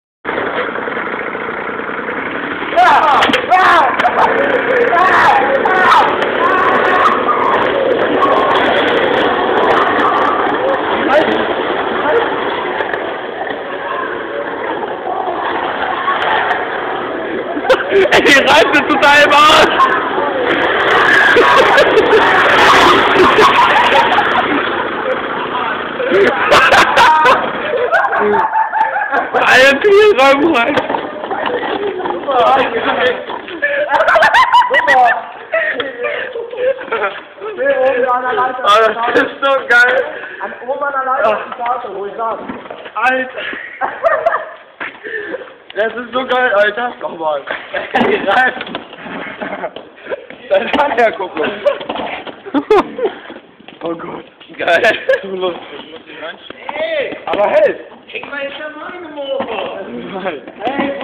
Kleiner Hänger
Jürgen hat endlich ein für sich passenden Hänger gefunden